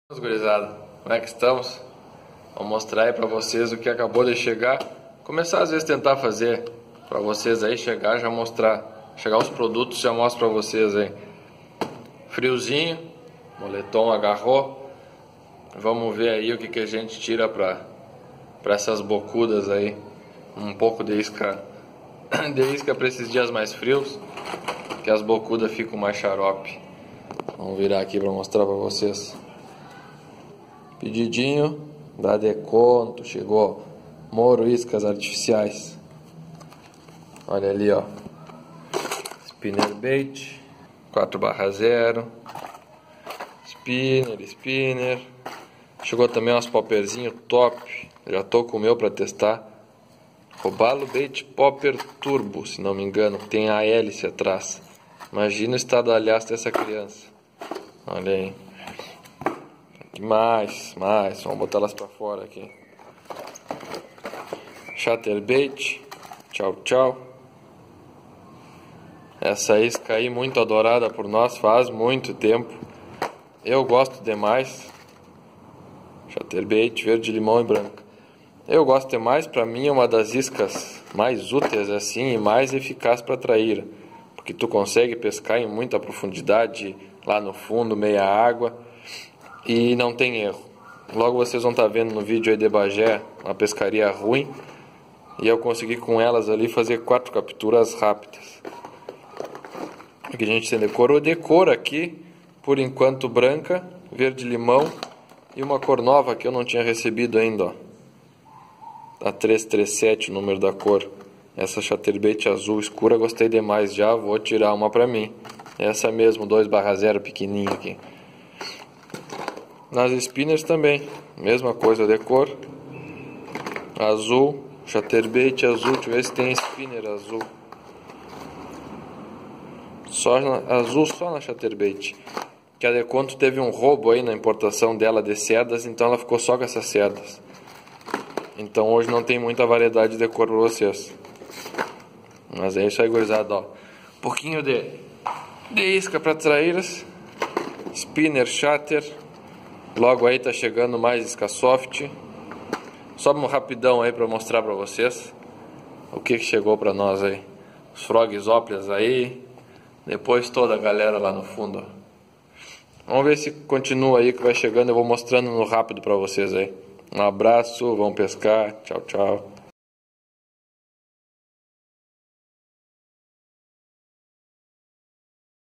Chegando mais iscas para trairas, no frio. Chatterbait e spinner bait. Loja Hoplias Pesca Esportiva
Ai pescadores. Umas das melhores iscas para trairas nessa temporada de águas frias e peixe manhoso. Iscas para epscaria de traira no frio, trairas no inverno.